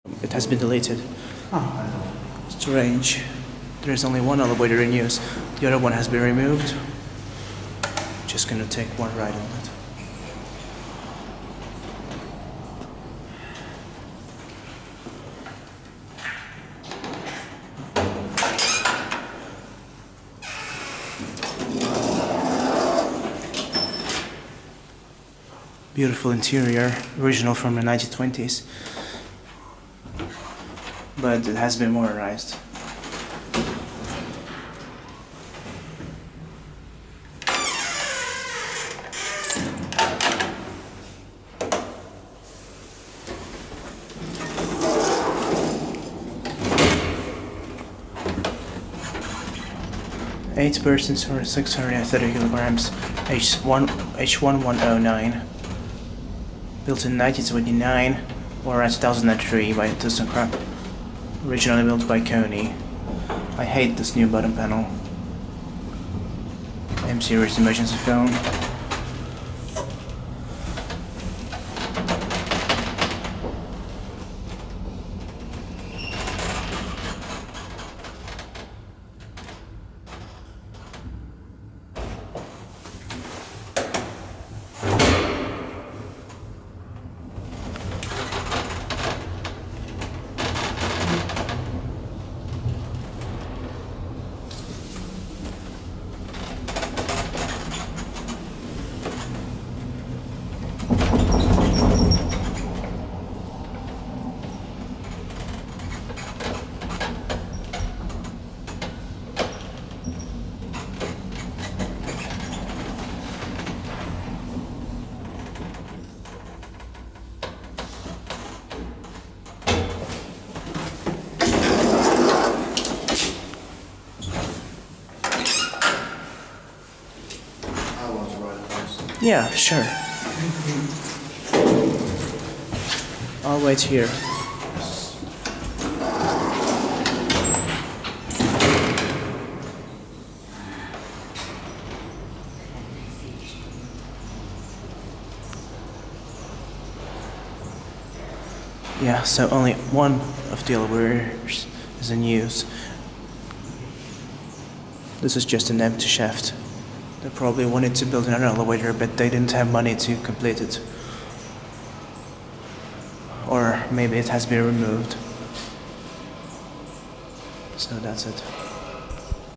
Old 1929 KONE (Mb ThyssenKrupp 2003) Tr. Elevator/Lift@Suomen Apteekkariliitto, Helsinki, Finland
Location: Suomen Apteekkariliitto (SWE: Finlands Apotekarförbund), Helsinki, Finland
Brand: KONE
Type: Traction
Modernized by: ThyssenKrupp (with generic fixtures)
Year installed/modernized: 1929 / 2003
Floors served: 6 (1, 2, 3, 4, 5, 6)
Capacity: 6 persons or 240 kg
Serial: H 1109
Note: I hate this modernization - the old button panel and call buttons were much nicer! There is space for another elevator, but it has either been removed, or no elevator was ever built (H 1110).